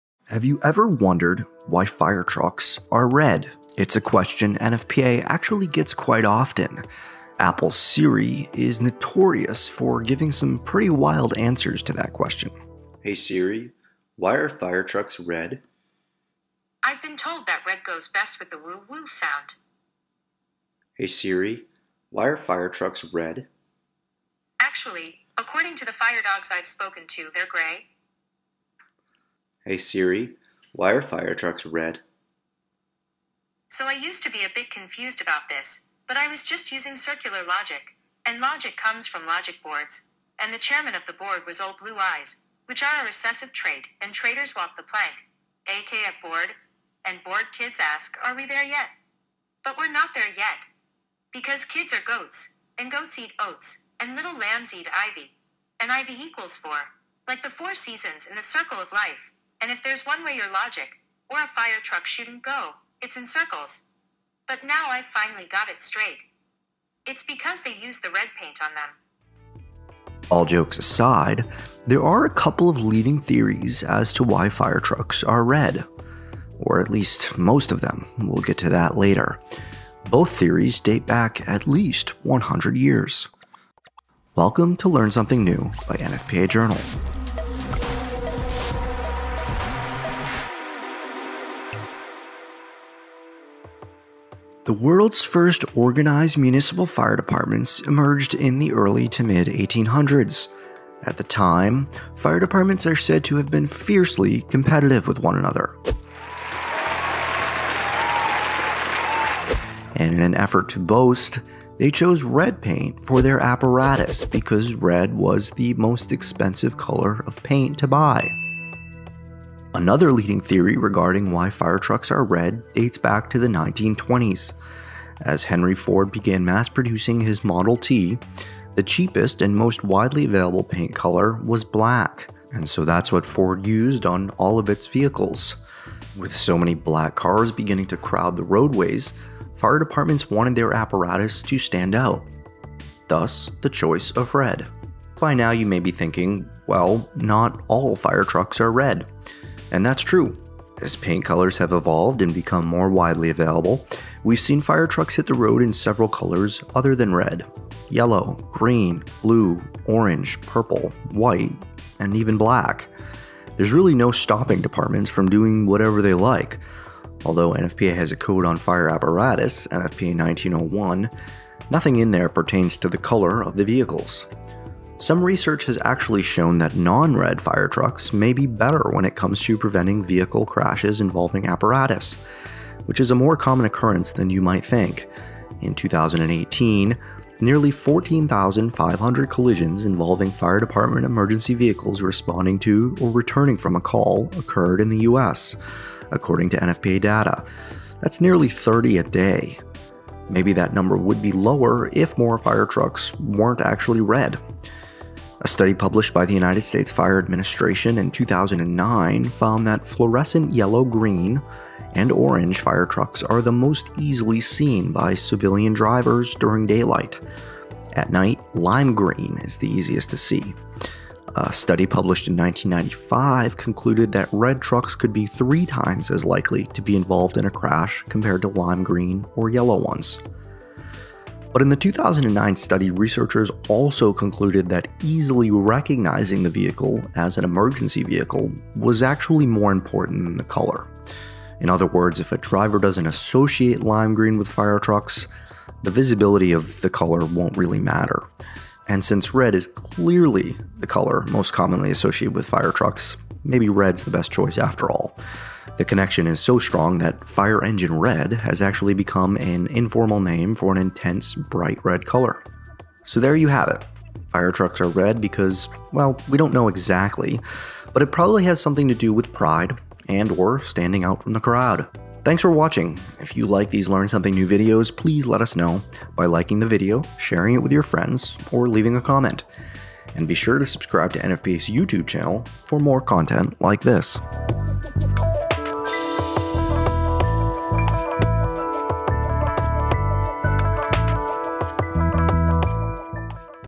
¿Por qué son rojos los camiones de bomberos?
A todos nos vendría bien un descanso del 2020. Entonces, en un enfoque más alegre de Aprenda Algo Nuevo de NFPA Journal®, este mes exploramos por qué la mayoría de los camiones de bomberos son rojos, así como la razón por la que el rojo puede no ser la mejor elección para vehículosde emergencia después de todo.